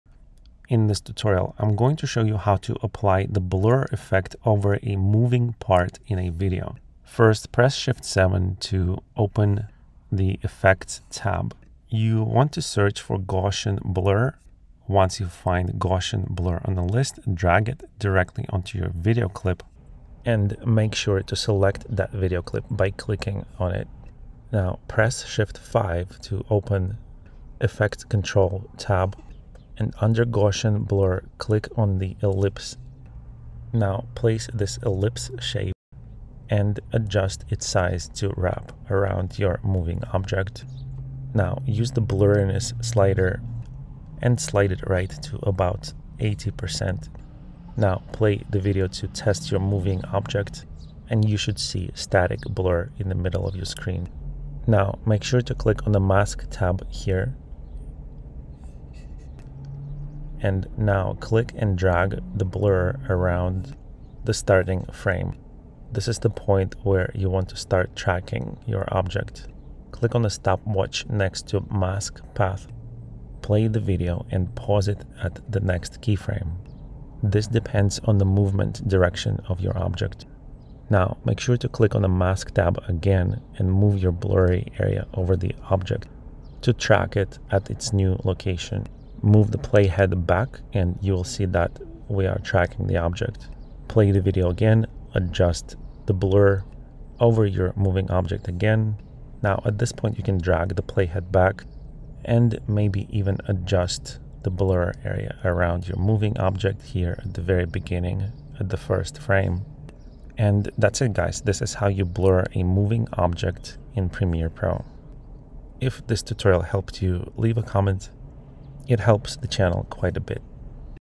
How To Blur Moving Object In Premiere Pro 2025
premierepro videoediting tutorial how to add blur effect to part of a video (circle, or rectangle) and animate blur around a moving object, motion tracking tutorial, track and follow an object using a blur effect.

Video Chapters:
00:00 Premiere Pro Blur Intro
00:07 Gaussian Blur
00:12 Apply Blur to Clip
00:23 Blur Area
00:49 Follow Moving Object
01:45 Results